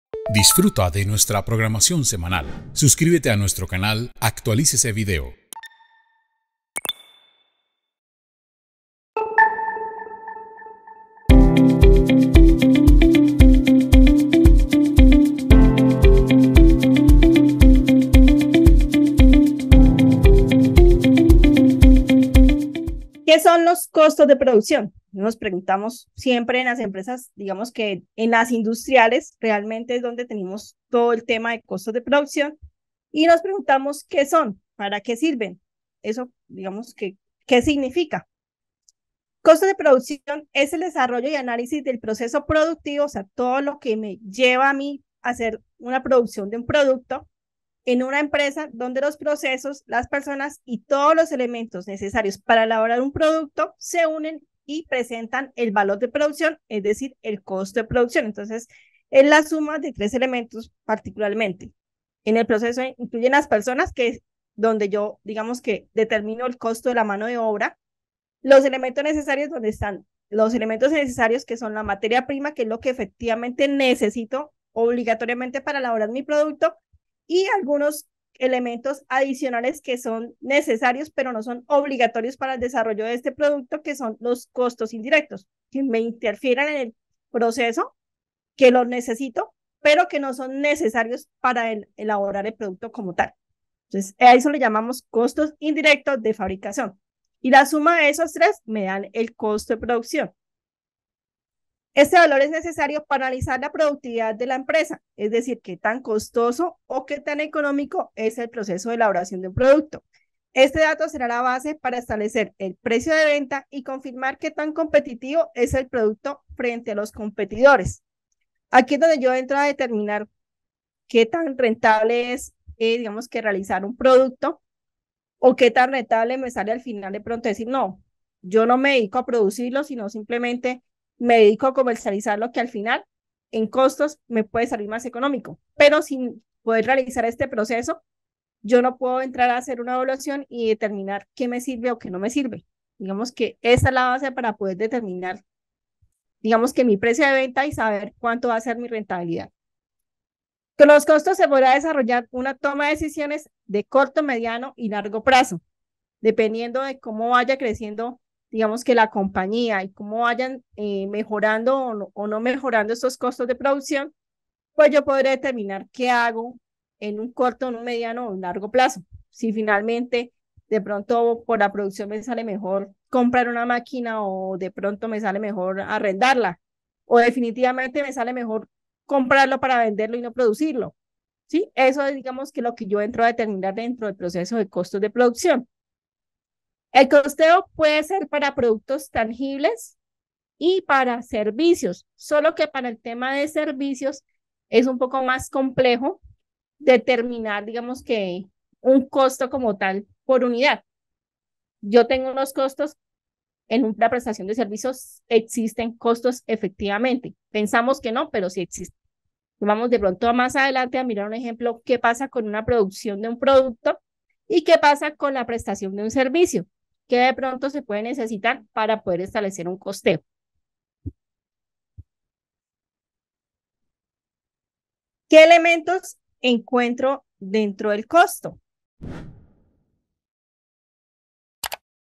Elementos del costo en servicios: fundamentos y caso práctico
🔥Accede a esta capacitación completa sobre Elementos del costo en servicios: fundamentos y caso práctico

Los costos de producción representan el análisis del proceso productivo, en el que tanto las personas como todos los elementos necesarios para elaborar un producto o prestar un servicio se integran para establecer el valor de producción. A partir de esta información, las empresas establecen el precio de venta.